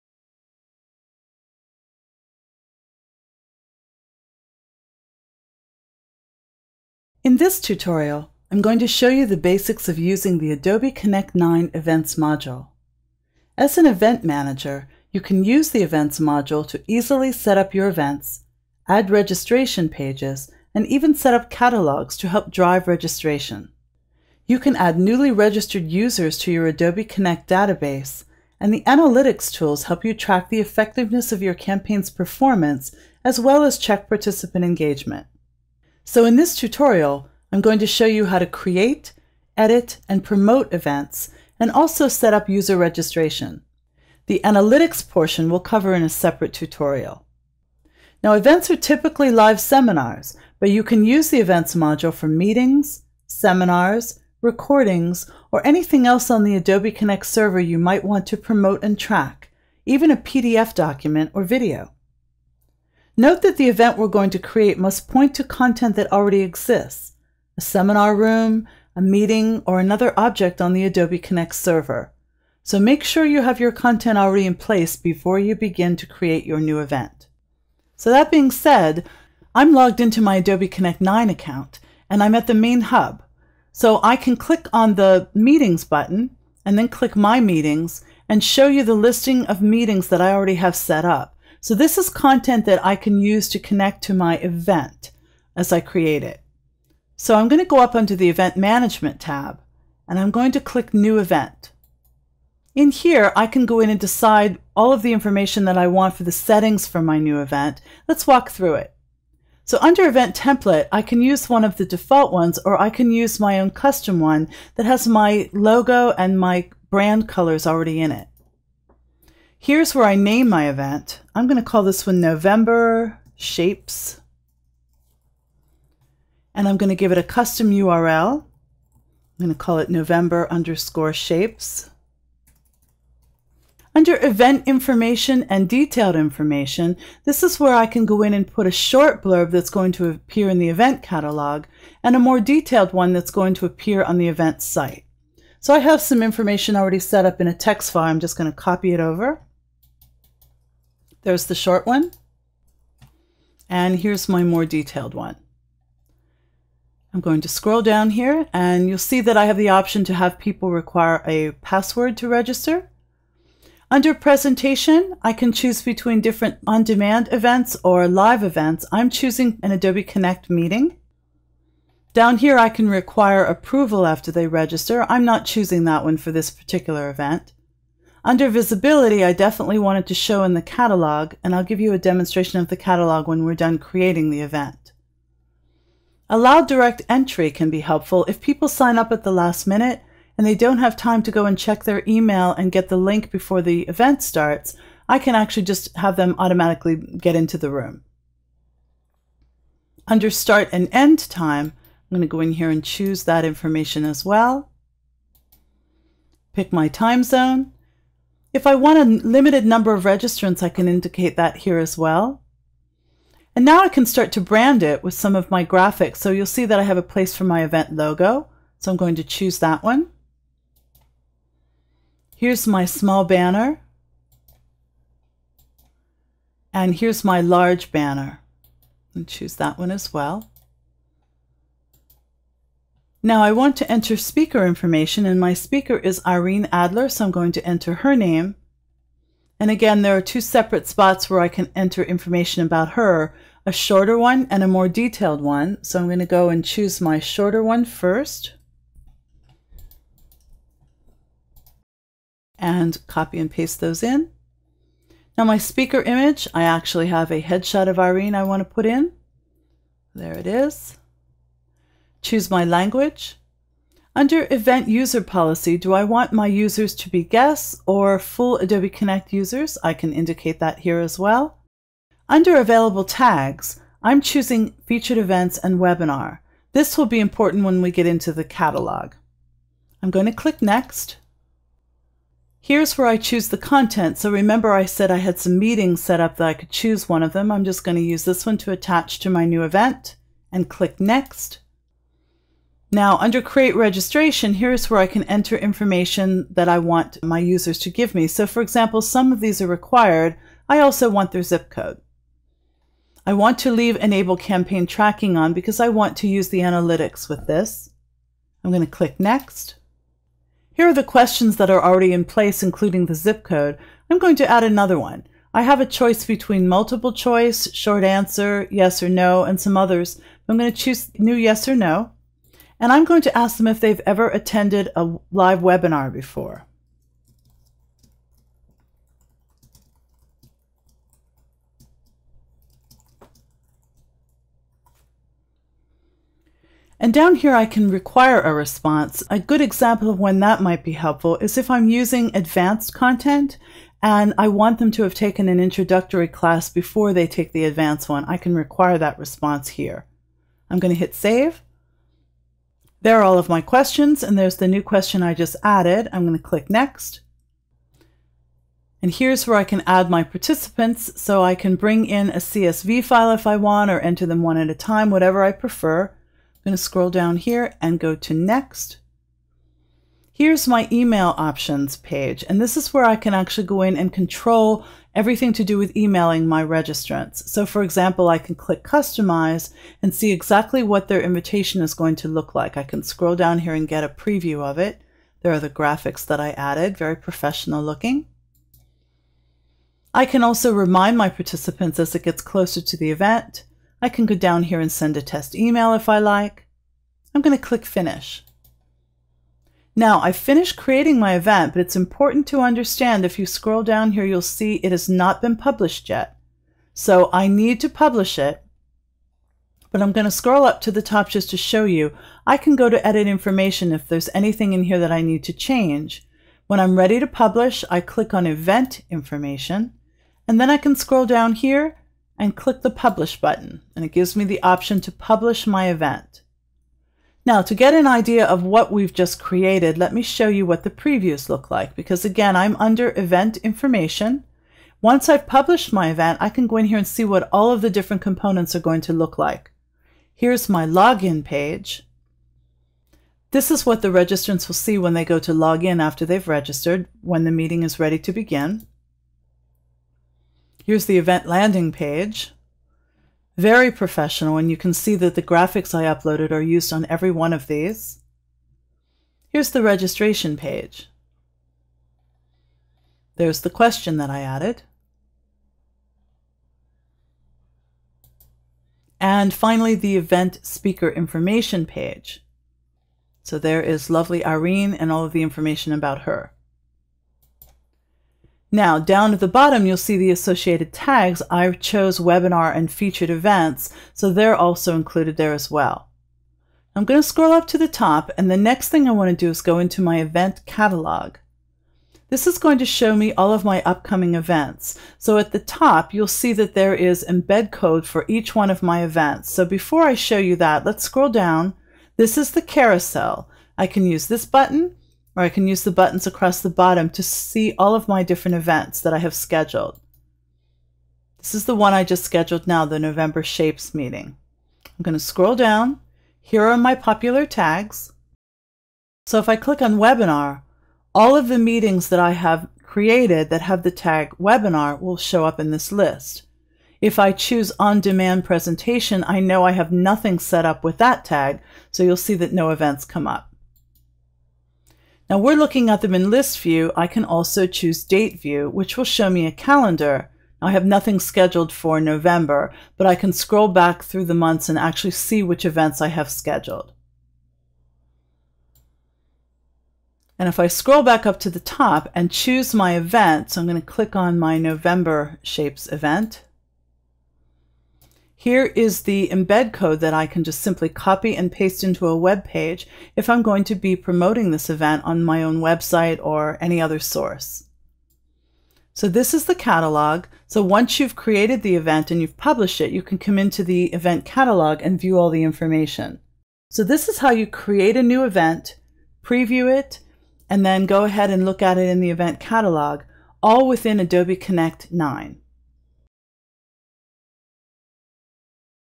Creating an event in Adobe Connect 9
This tutorial will demonstrate how to create an event in Adobe Connect 9 and add it to the event catalog